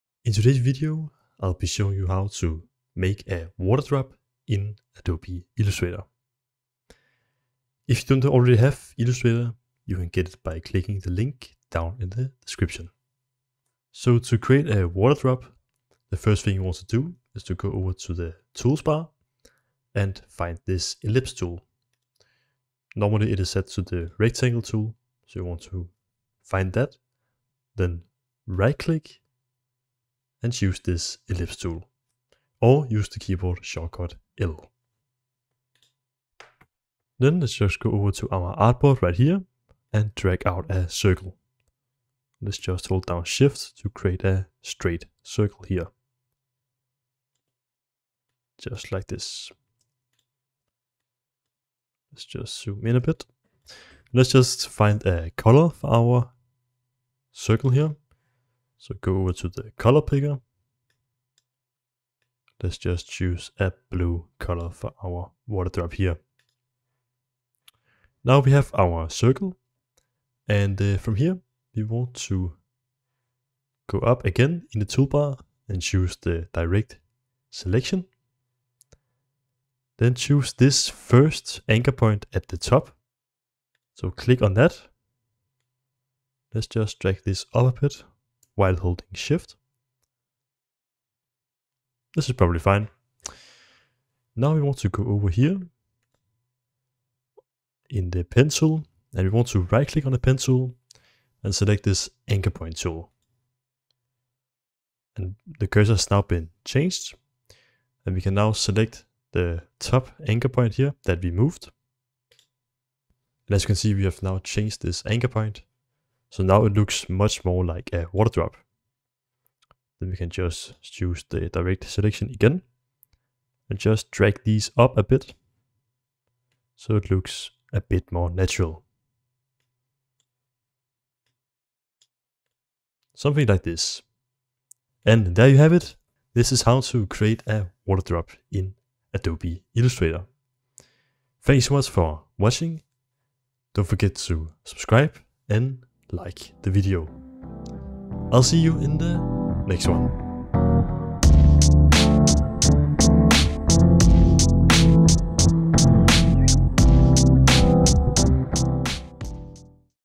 How to Make a Water Drop In Adobe Illustrator
In today's video, I'll be showing you how to make a water drop in Adobe Illustrator